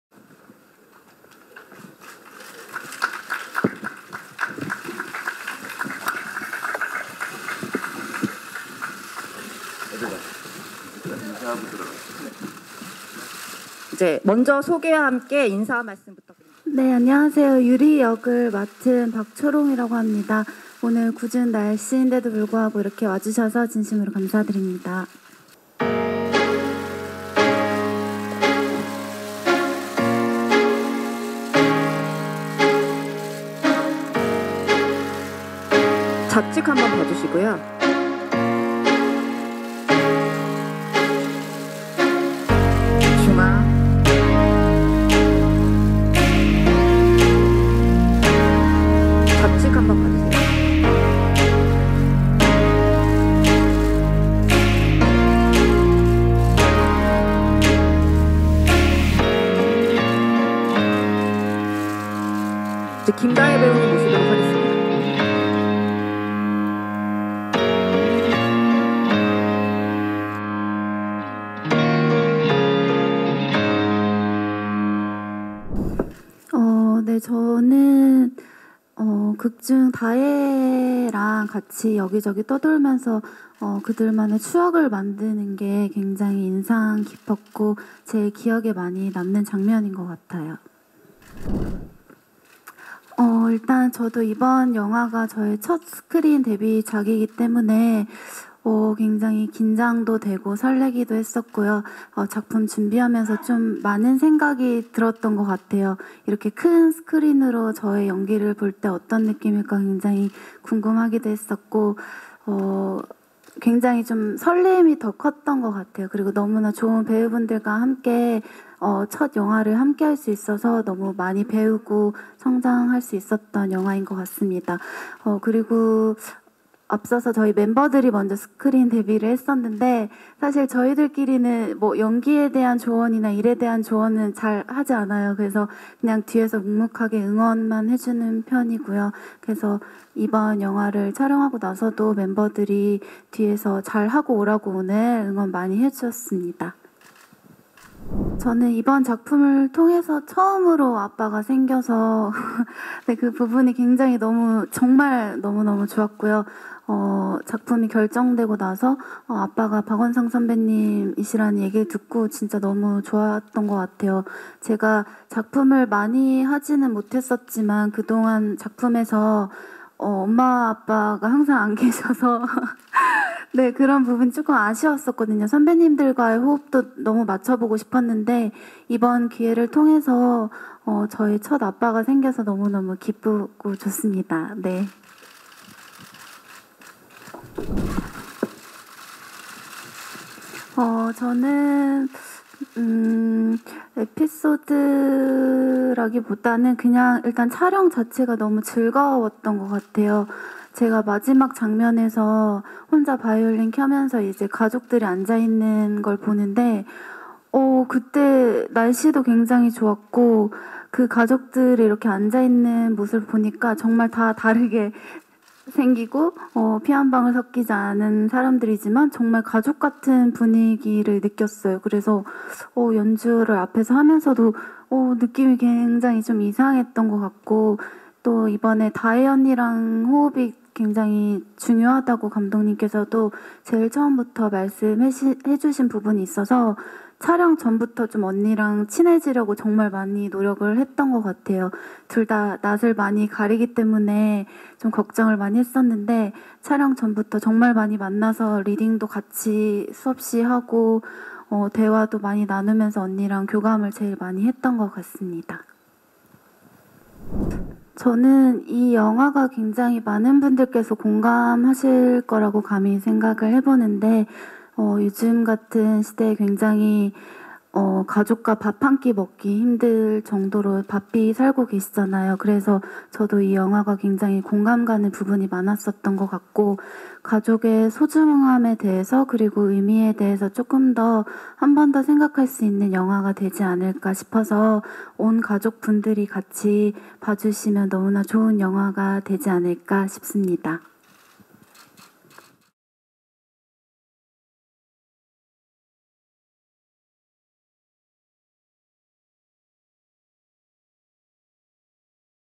에이핑크 (Apink) 초롱, 스크린 데뷔 '불량한 가족' 언론배급시사회 ★
24일 오후 서울 광진구 롯데시네마 건대입구에서 영화 '불량한 가족'(감독 장재일) 언론배급시사회가 열렸다. 시사 후 진행된 기자간담회에는 배우 박원상, 박초롱, 김다예 그리고 장재일 감독이 참석했다.